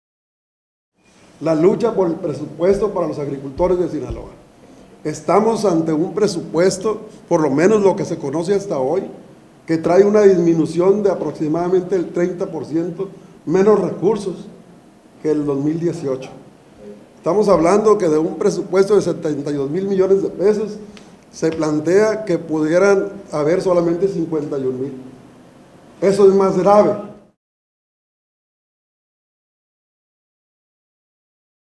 RECHAZAN ELIMINACIÓN CUOTAS
Las organizaciones del CAP rechazan que se elimine cuota liga o ejidal